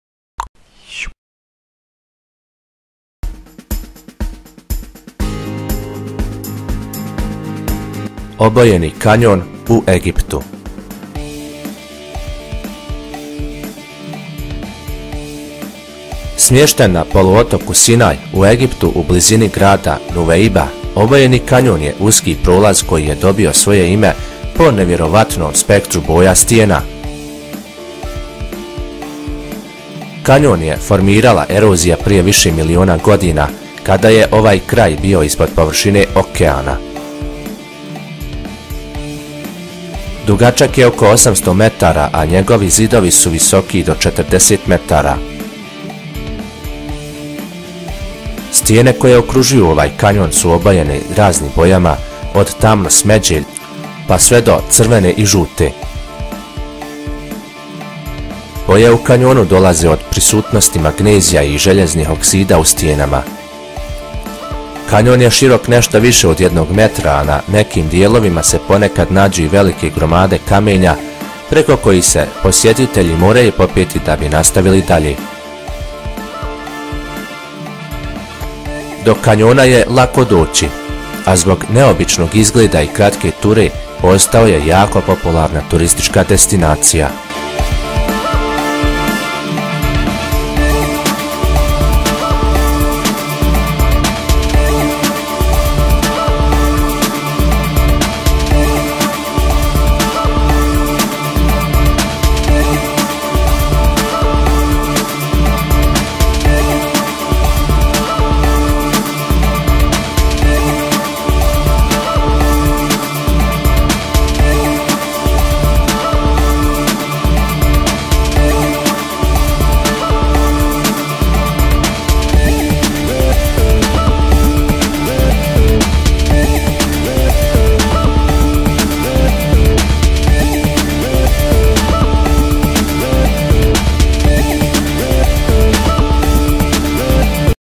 Obojeni kanjon u Egiptu
Smješten na poluotoku Sinaj u Egiptu u blizini grada Nuweiba, Obojeni Kanjon je uski prolaz koji je dobio svoje ime po nevjerovatnom spektru boja stijena. Kanjon je formirala erozija prije više miliona godina, kada je ovaj kraj bio ispod površine oceana.

Dugačak je oko 800 metara, a njegovi zidovi su visoki i do 40 metara. Stijene koje okružuju ovaj kanjon su obojene raznim bojama, od tamno smeđe, pa sve do do crvene i žute. Boje u kanjonu dolaze od prisutnosti magnezija i željeznih oksida u stijenama.

Kanjon je širok nešto više od jednog metra, a na nekim dijelovima se ponekad nađu i velike gromade kamenja preko kojih se posjetitelji moraju popeti da bi nastavili dalje. Do kanjona je lako doći, a zbog neobičnog izgleda i kratke ture postao je jako popularna turistička destinacija.